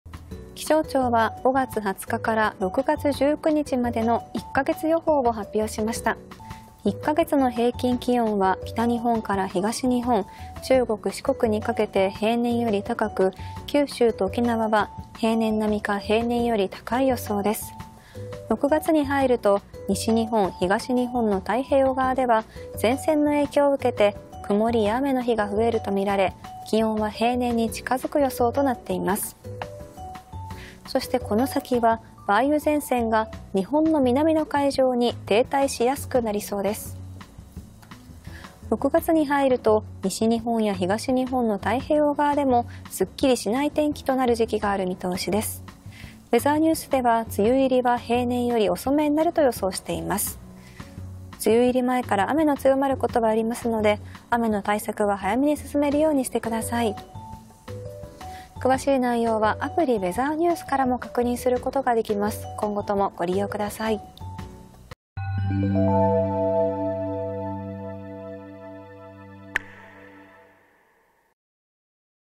気象庁1か月予報 6月に入ると西日本や東日本にも段々と梅雨の気配
18日(木)、気象庁は5月20日から6月19日までの1か月予報を発表しました。日本の南から暖かな空気が流れ込みやすく、気温は平年よりも高い所が多い予想となっています。次第に梅雨前線が停滞しやすくなり、西日本、東日本でも梅雨の気配がみえてきそうです。